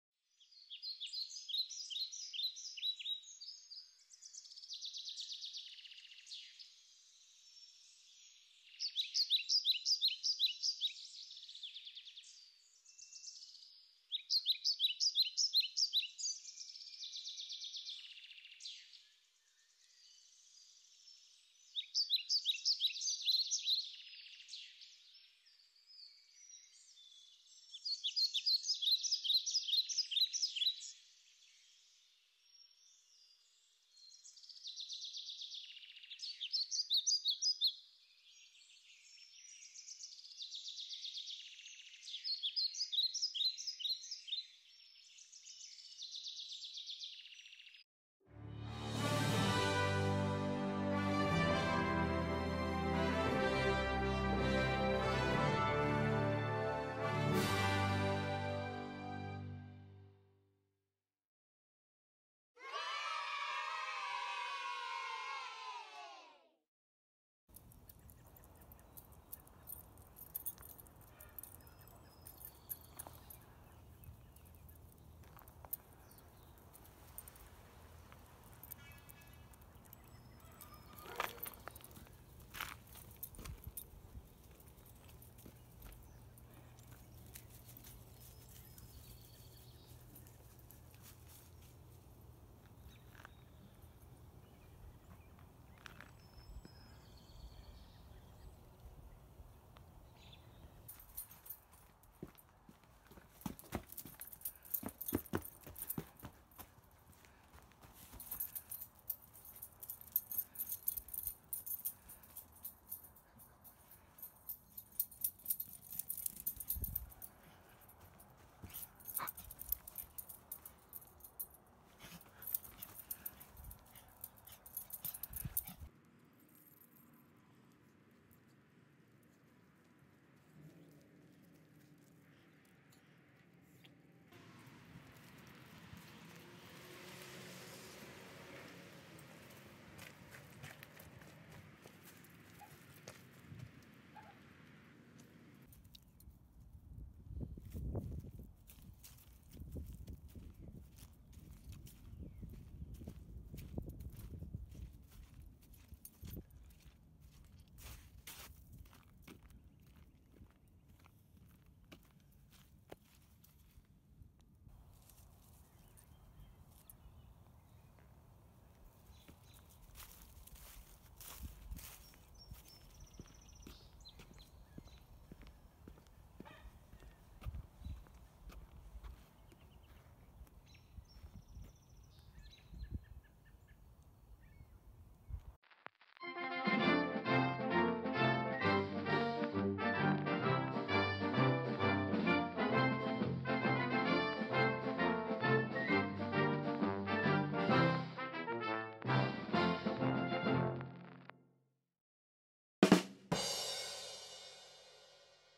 Eddie goes to Stonehaven
a chihuahua visits a retirement home